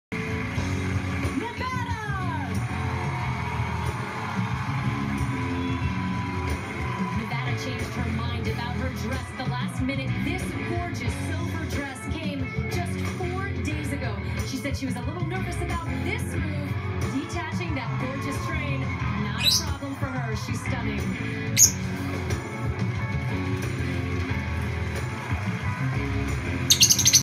My Joey Galon, ATELIER gown on the Miss USA stage
My custom designed Joey Galon, ATELIER gown on the televised Miss USA stage that I designed for Miss Nevada, Brittany McGowan. She placed 2nd runner up.

*I always wanted on a gown at Miss USA...dreams really do come true! Next stage to design for: Miss Universe! 😊🙏🏼